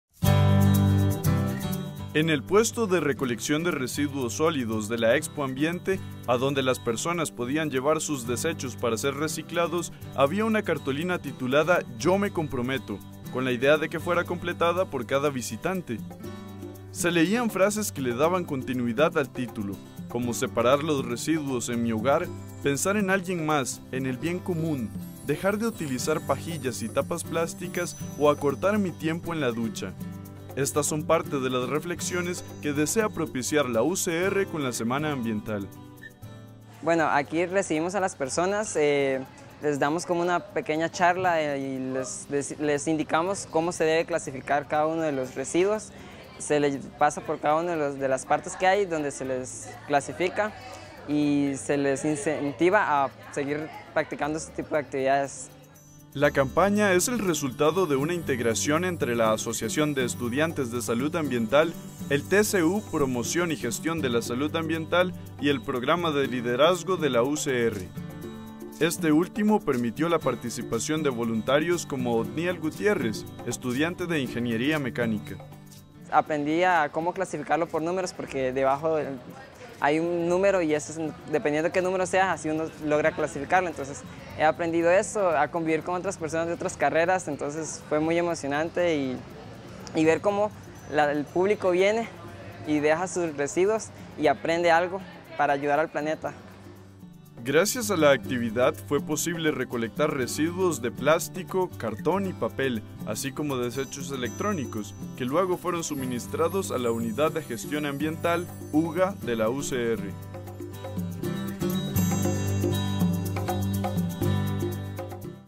Reuso y recolección de desechos - Semana Ambiental
Reciclar va más allá de darle un nuevo uso a los materiales de desecho. También es una oportunidad para que respetemos el medio ambiente y nos relacionemos de manera armoniosa con este a través de eventos como la Semana Ambiental UCR 2017, que celebró su segunda edición con el lema "UCR te conecta con la naturaleza"; y desarrolló actividades como la recolección de residuos sólidos.